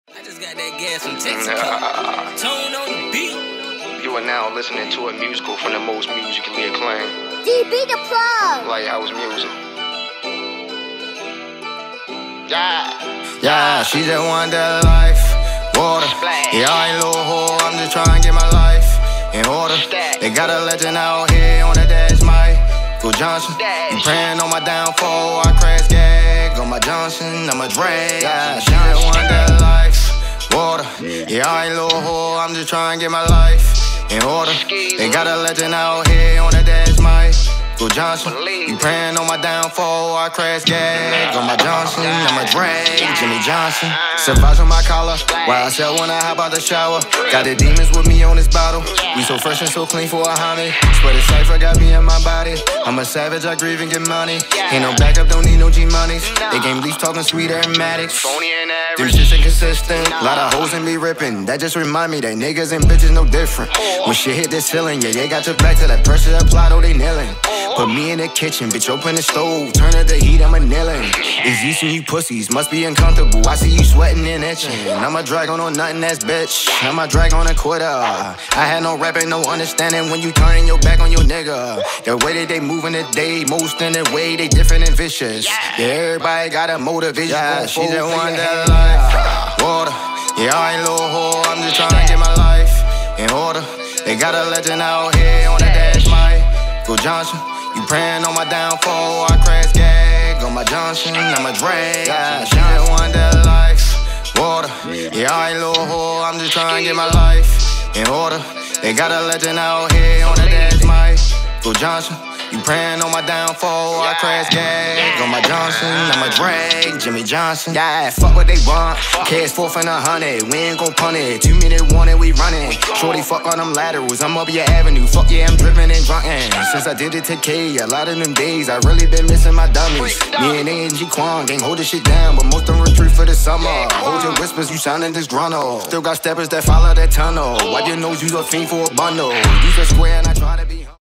Pheno Vein - YSL Talent Search Tour Recap (wk 3)
PhenoVein burns the road up again!!! This weekend the YSLTalentSearchTour makes it’s stops thru Macon, GA, Birmingham, & Montgomery, AL. YSL anchors the tour with their newly signed artist yakgotti !!!
🎥 • footage shot and edited by @phenovein